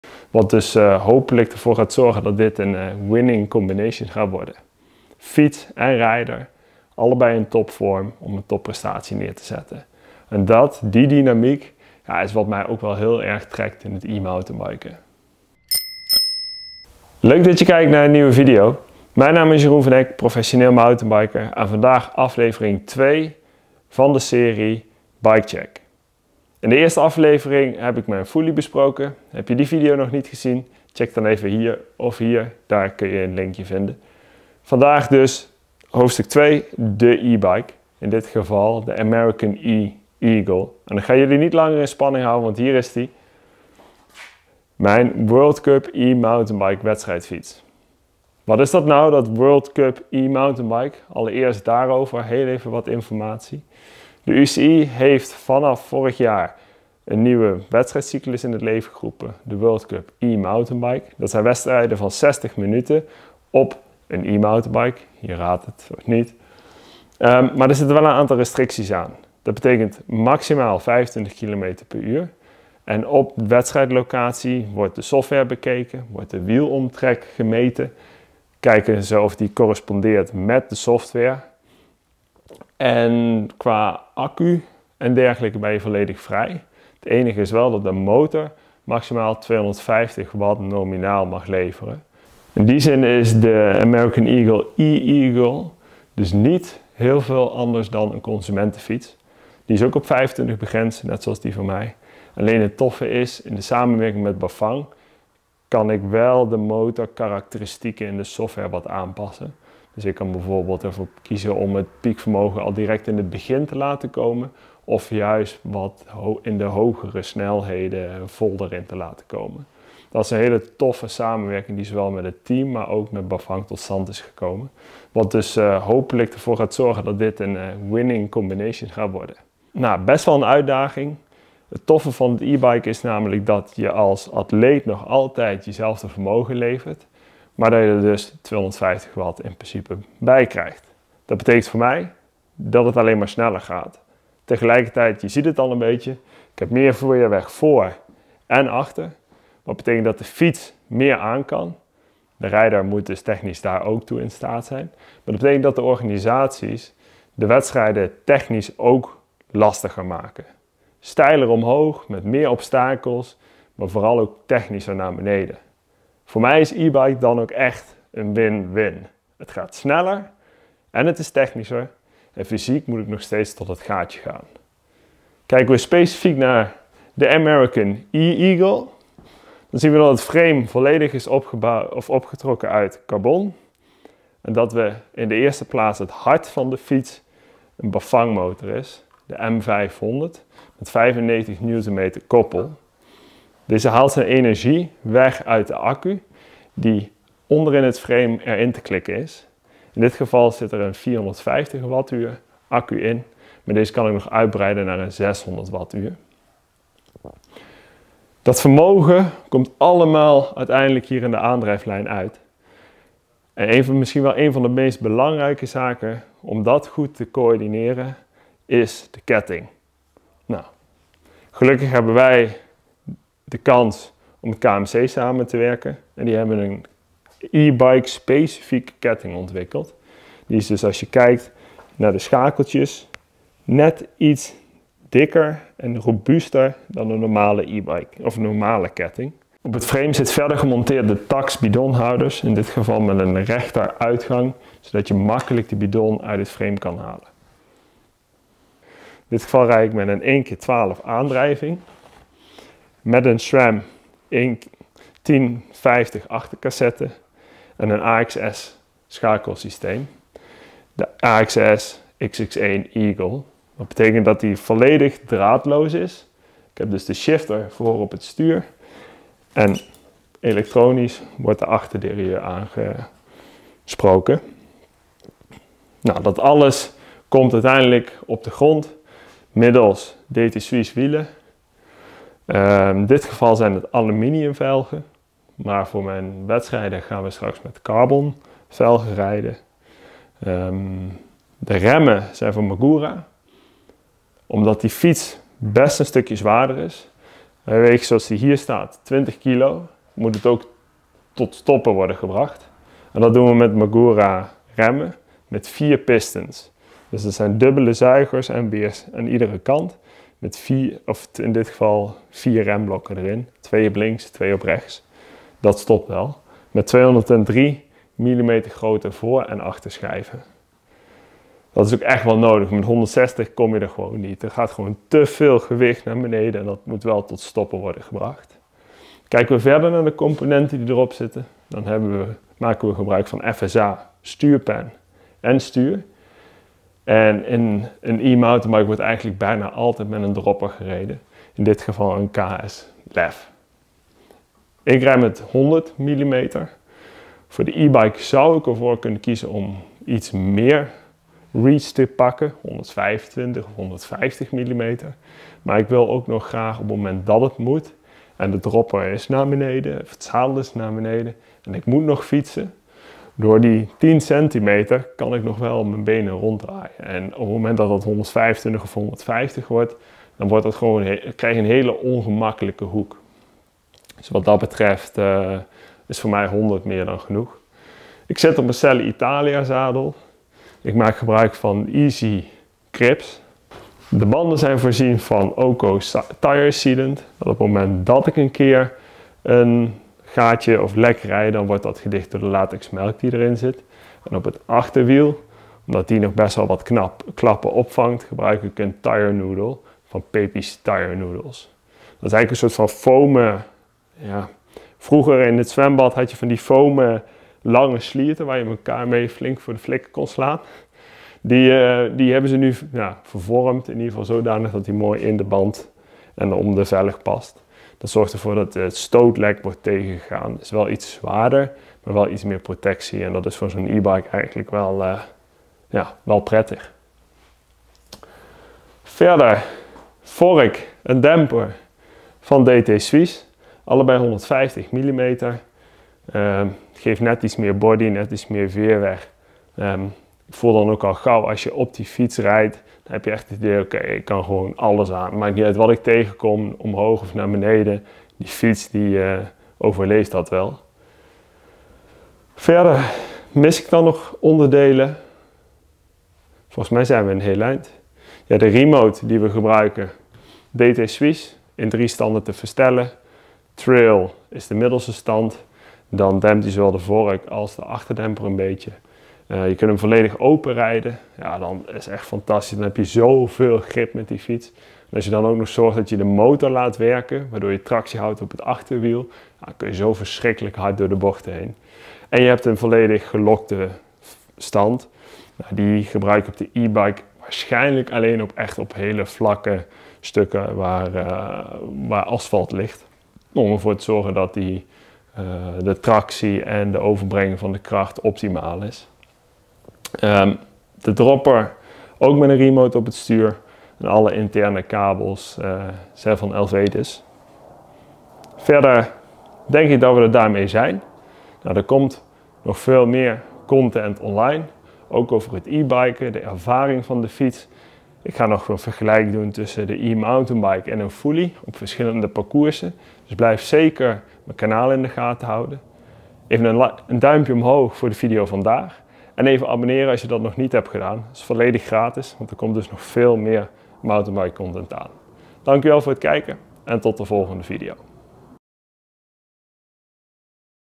BikeCheck The American Eagle E-agle World Cup E-MTB bike
Een nieuw team betekent nieuwe fietsen. In de serie BikeCheck neem ik je in drie afleveringen mee in mijn CST PostNL Bafang MTB Racing Team wedstrijd fietsen voor het seizoen 2021.

Aflevering twee draait om electric power. De American Eagle E-agle, mijn wedstrijdfiets voor de UCI World Cup E-MTB races.

De gedetailleerde lijst met componenten vind je hieronder:
Frame: American Eagle E-agle 29” size 19.5
Engine: Bafang M500 95nm & 250W
Battery: Bafang BT F03 450 wh
Fork: DT Swiss F535 150mm
Shock: DT Swiss R535 150mm
Tires: CST
Wheels: DT Swiss EX 1701 Spline
Cranks: Bafang + 38T round
Chain: KMC e12 Ti-N Gold
Cassette: Sram XG-1299 Eagle 10-50
Derailleur: Sram XX1 Eagle AXS
Brakes: Magura MT5 203mm
Stem: FSA SCR 80mm
Handlebar: FSA KFX flat 760mm
Seatpost: FSA K-Force
Dropperpost: KS LEV-Ci
Saddle: Selle Italia SLR Boost kit Carbonio
Grips: Esi Chunky
Cables: Elvedes
Tyre Sealant: OKO
Tyre Noodle: Pepi's Tire noodle

Tyre repair: Dynaplug Racer
Tools: SKS Germany
Lubricant: Squirt lube wax
Bottles & cages: Tacx Radar
Nutrition: Cadence nutrition

Camerawerk en edit: Ivo Kerckhoffs